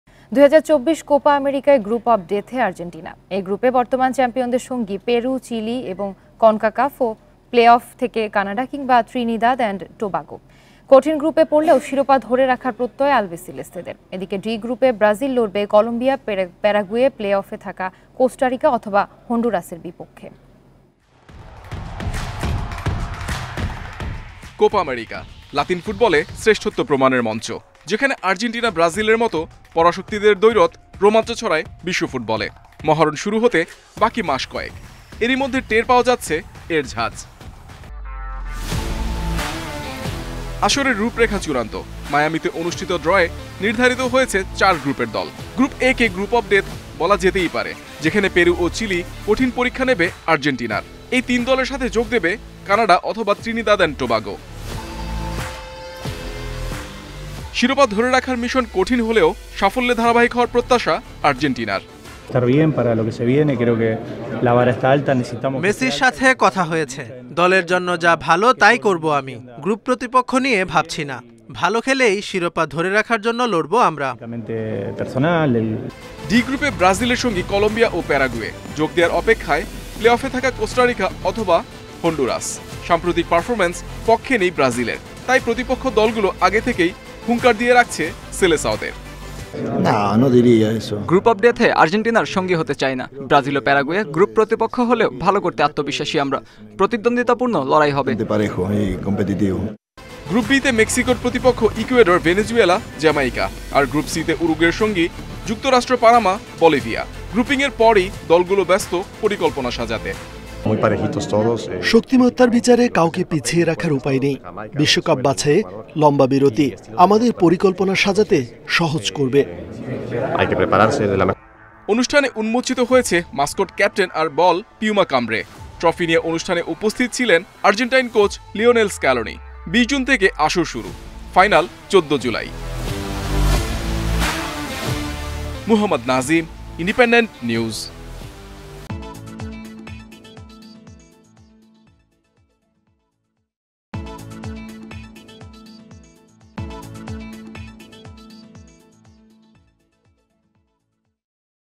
আর্জেন্টিনা পারবে শিরোপার ধারাবাহিকতা ধরে রাখতে?। Sports
২০২৪ কোপা আমেরিকায় গ্রুপ অব ডেথে আর্জেন্টিনা। এ গ্রুপে বর্তমান চ্যাম্পিয়নদের সঙ্গী পেরু, চিলি এবং কনকাকাফ প্লে-অফ থেকে কানাডা কিংবা ত্রিনিদাদ এন্ড টোবাগো। কঠিন গ্রুপে পড়লেও শিরোপা ধরে রাখার প্রত্যয় আলবিসেলেস্তেদের। এদিকে, ডি গ্রুপে ব্রাজিল লড়বে কলম্বিয়া, প্যারাগুয়ে, প্লে-অফে থাকা কোস্টারিকা অথবা হন্ডুরাসের বিপক্ষে।

Argentina in Group of Death in 2024 Copa America. In this group, the current champions Peru, Chile and Canada or Trinidad and Tobago from the CONCACAF playoffs. Albiceleste are determined to retain the title despite falling into a tough group. Meanwhile, Brazil will face Colombia, Paraguay, playoff contenders Costa Rica or Honduras in Group D.